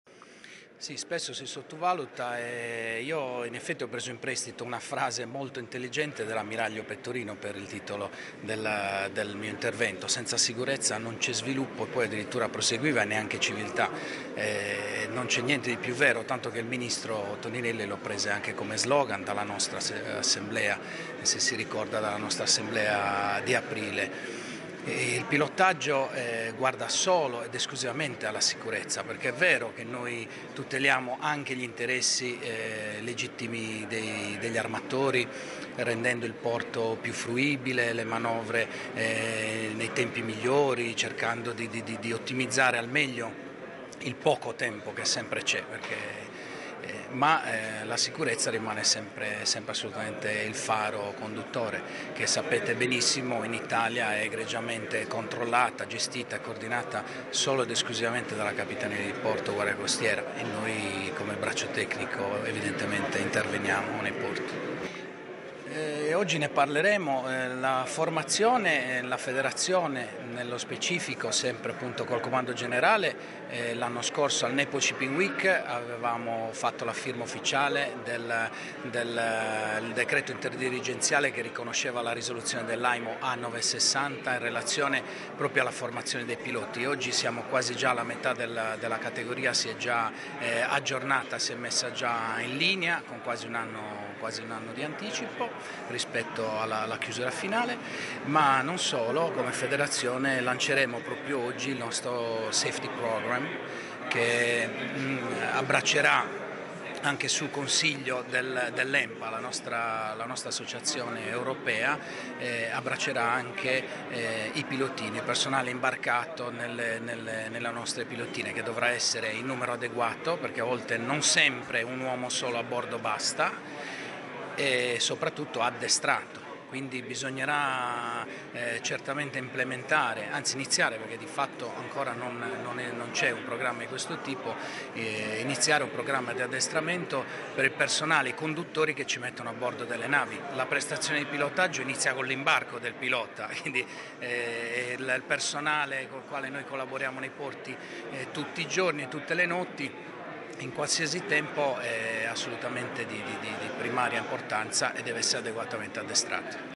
Francesco Bandiera, presidente Federazione italiana piloti dei porti
Senza sicurezza non c'è sviluppo. Fedepiloti lo dice forte in una sessione dedicata alla maritime safety. Fondamentale puntare sulla formazione, anche di coloro che accompagnano i piloti sotto la nave